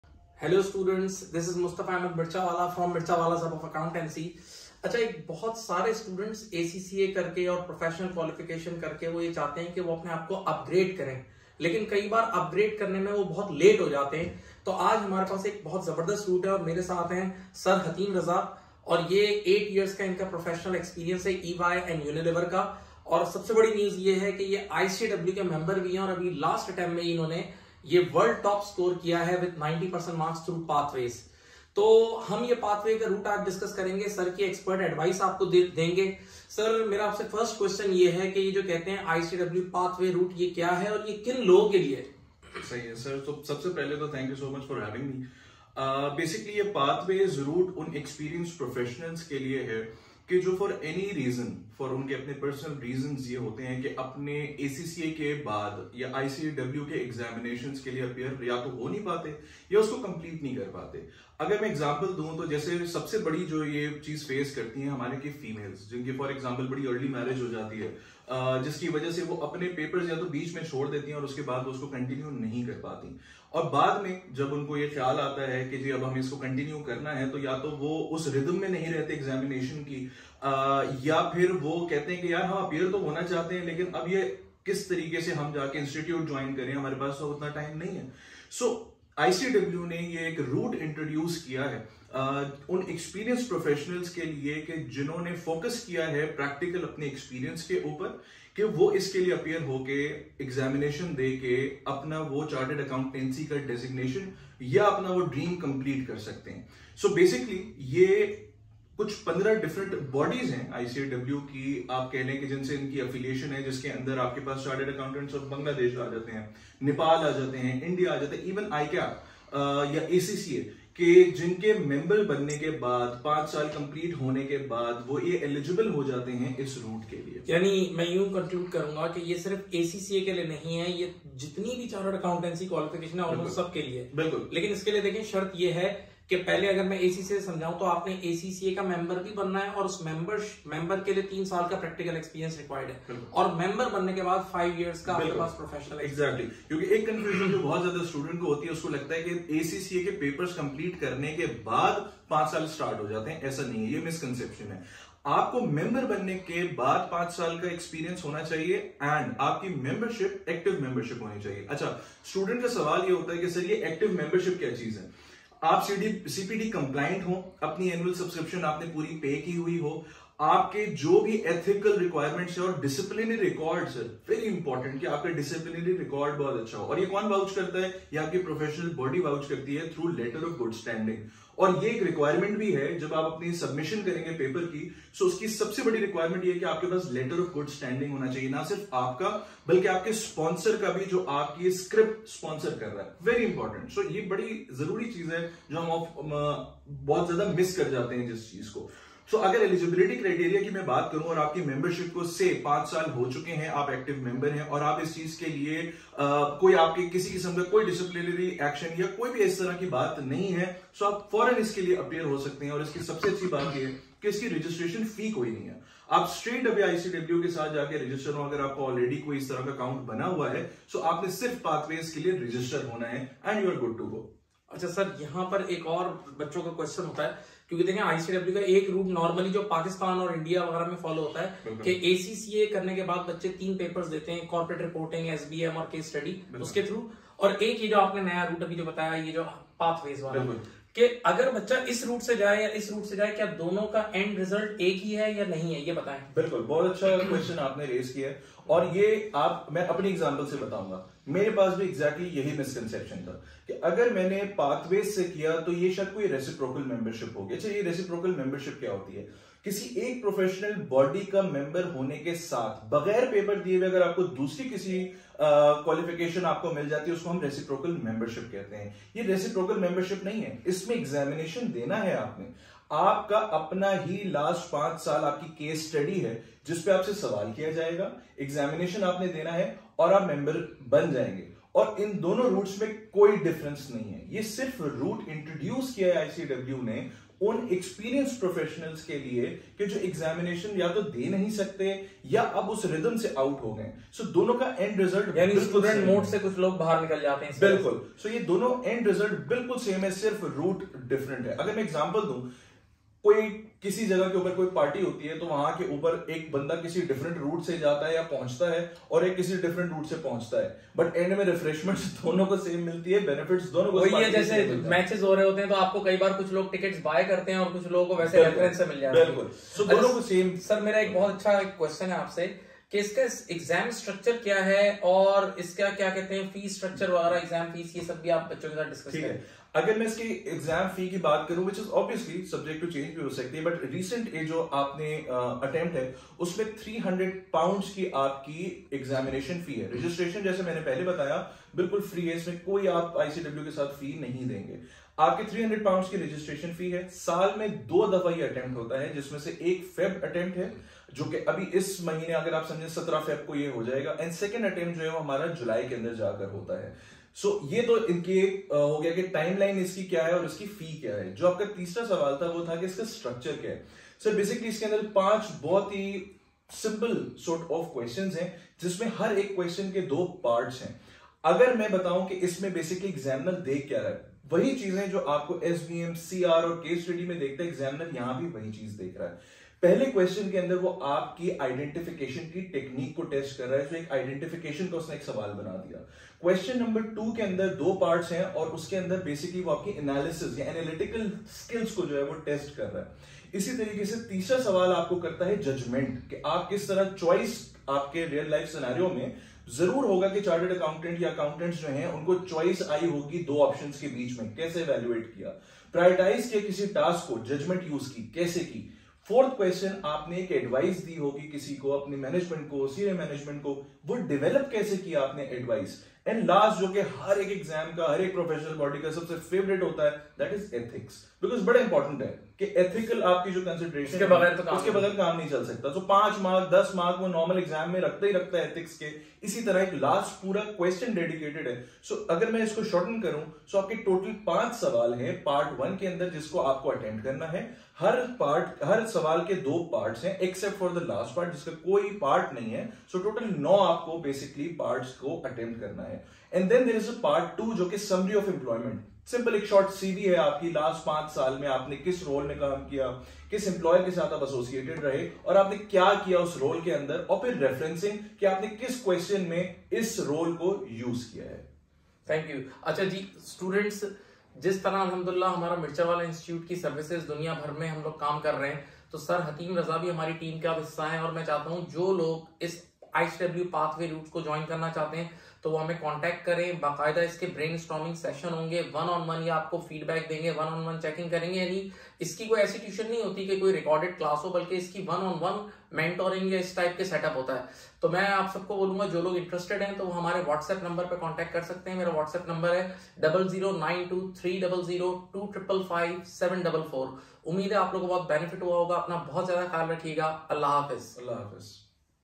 All About ICAEW PATHWAYS ROUTE. Talk by Mustafa_Mirchawala
All About ICAEW PATHWAYS ROUTE. Talk by Mustafa Mirchawala (CEO Mirchawala's Hub of Accountancy)